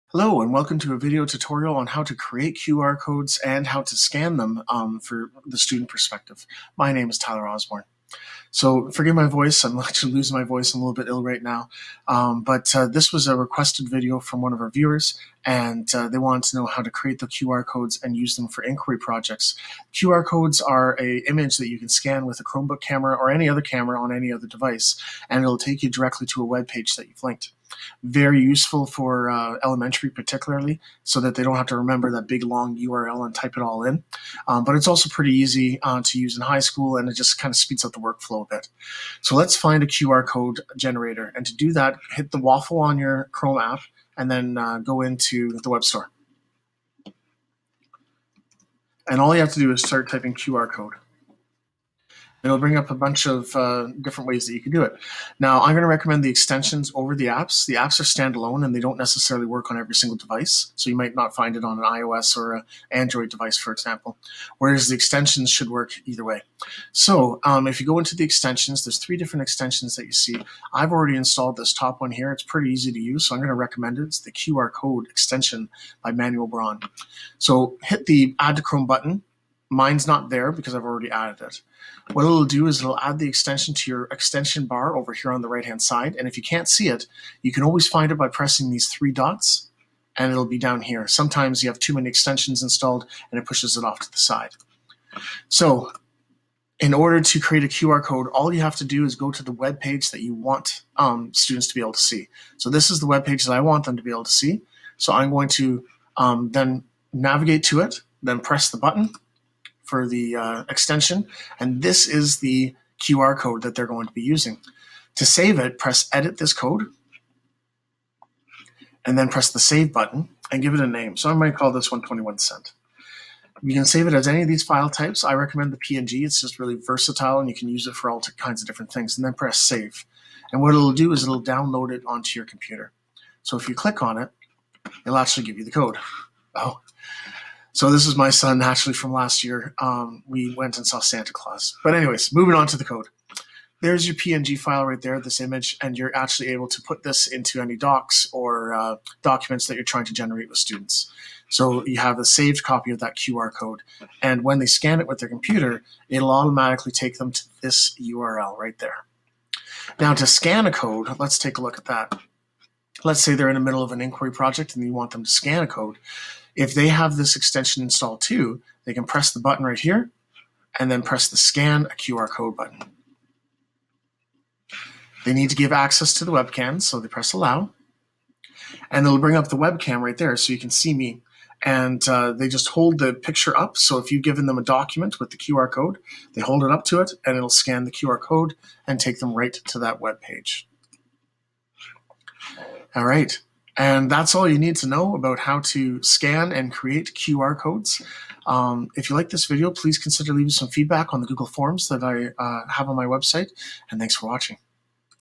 How to create and scan QR Codes with a Chrome extension
This short tutorial will show you how to create and scan QR codes with a Chrome extension. This is particularly useful for helping younger students who have difficulty copying an entire URL.